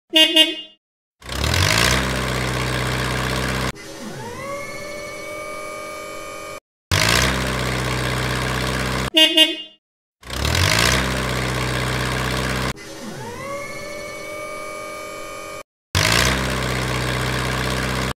Forklift 3D Model - Cinema 4D
A basic 3D model of a forklift picking up a box made in Cinema 4D.

A scene made to learn the basics of Cinema 4D; modelling, animation, rendering settings and lighting.

Also, analysing the difference between Autodesk Maya and Cinema 4D